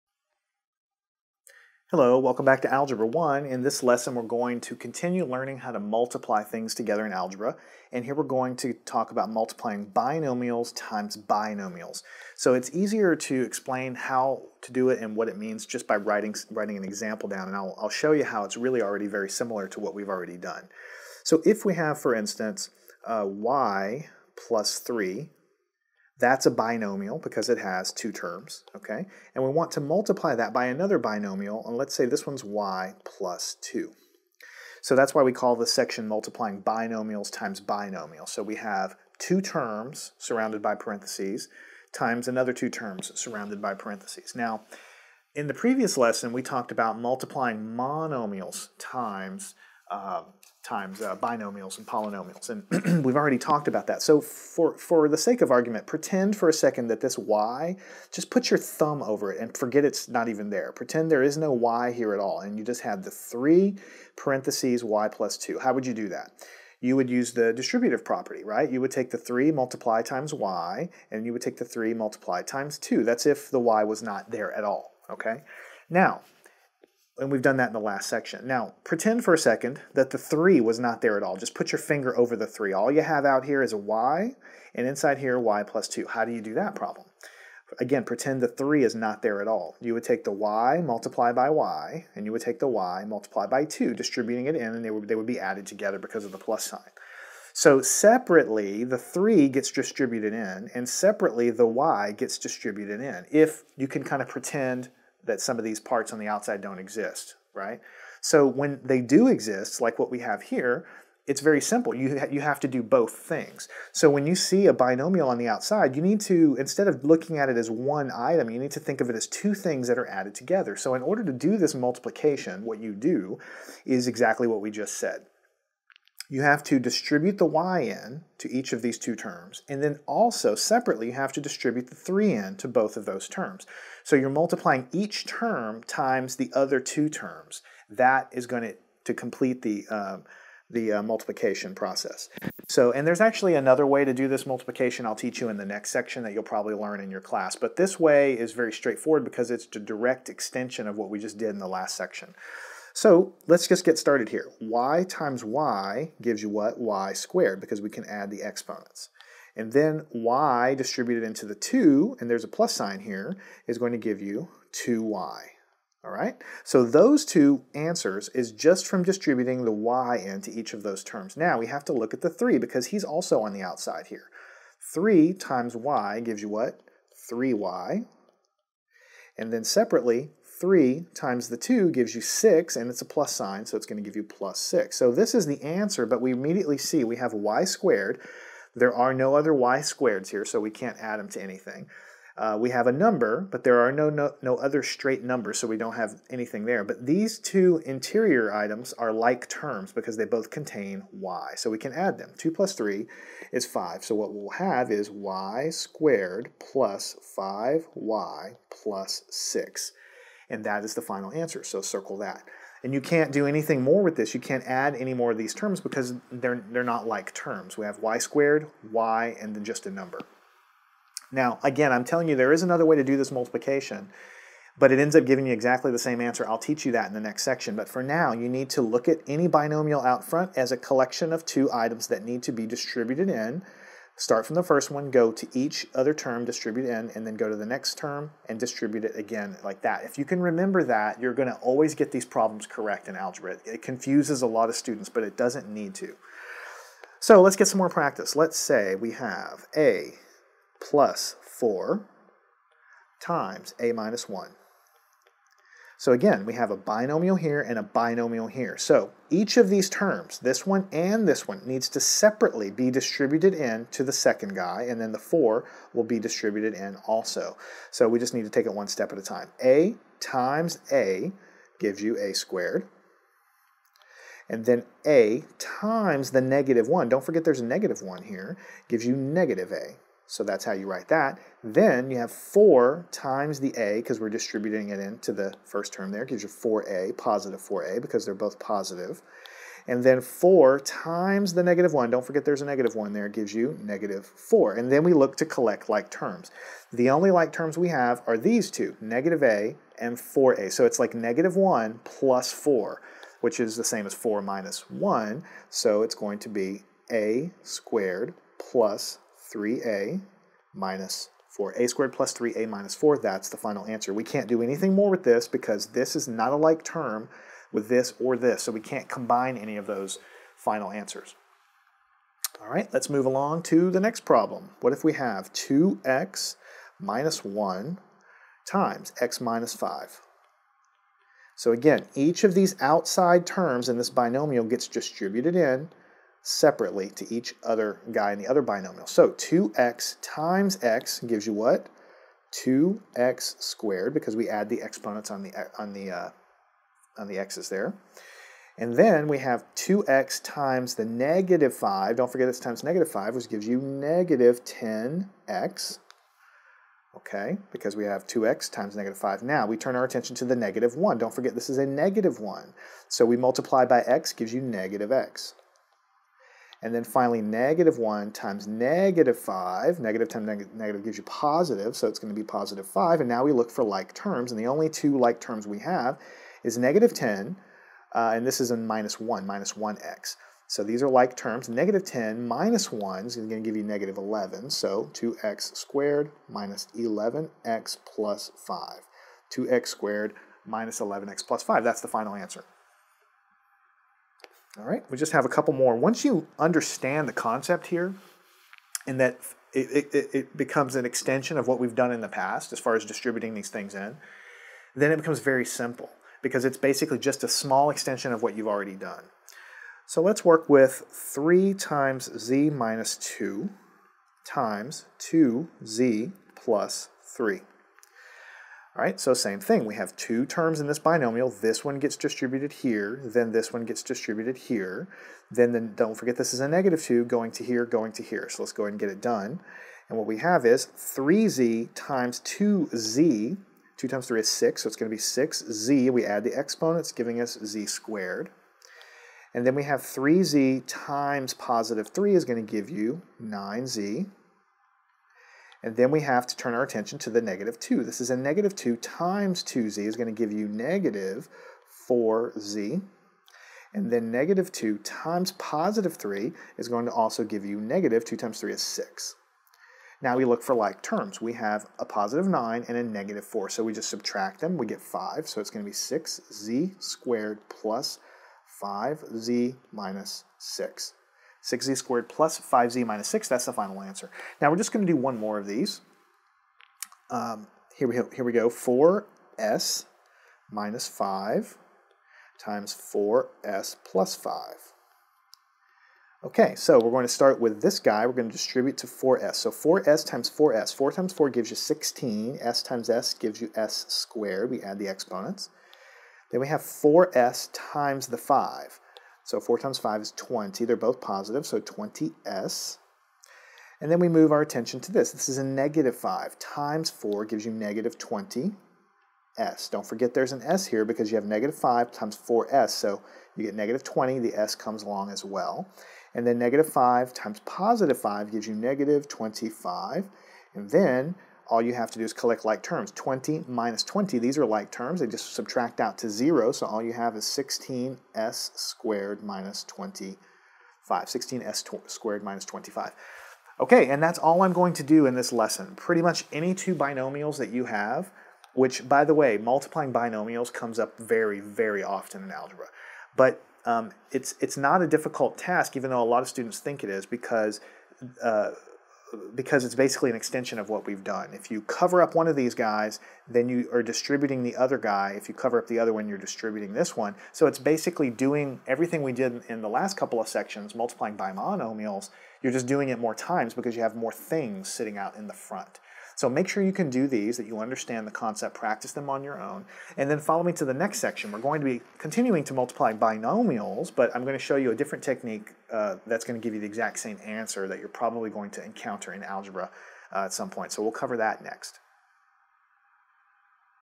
16 - Multiply Binomials by Binomials in Algebra (Learn Binomial Multiplication)
Learn how to multiply a binomial by a binomial in algebra by performing the multiplication term by term.  First we distribute in the first term and do the first two multiplications.  Then, we distribute in the second term and do the final two multiplications.  Lastly, we simplify the results.